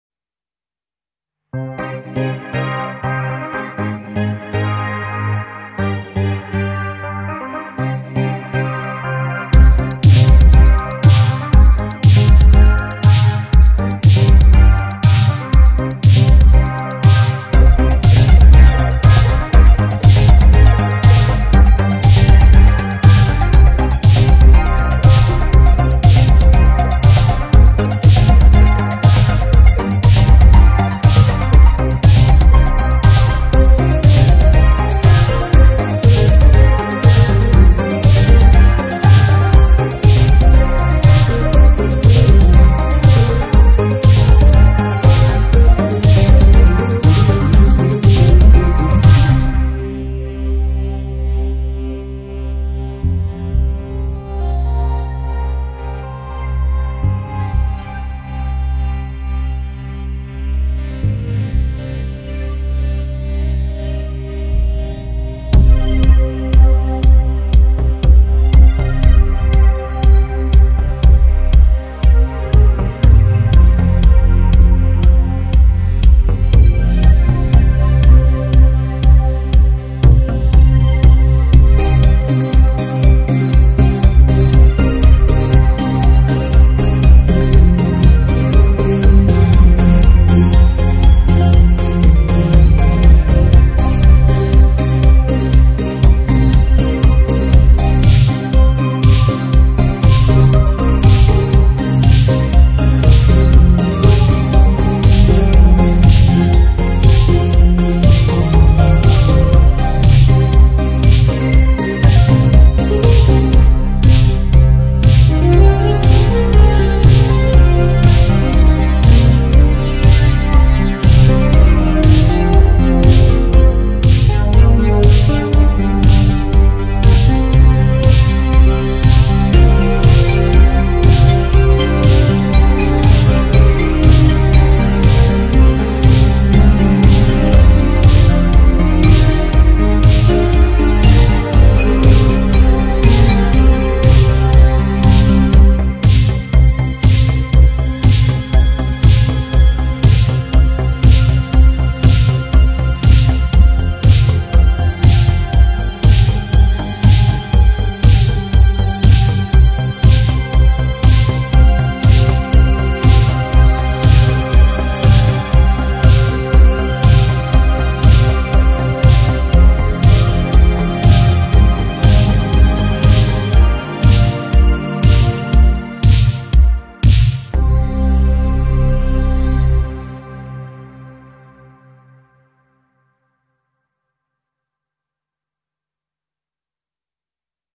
Attack on Titan Music Video -- Anime-inspired synthwave music by Sean Harrigan /copyright free music
This is a musical homage to the TV series Attack on Titan. The music is composed by me and is copyright free. Anyone may use it as they would like.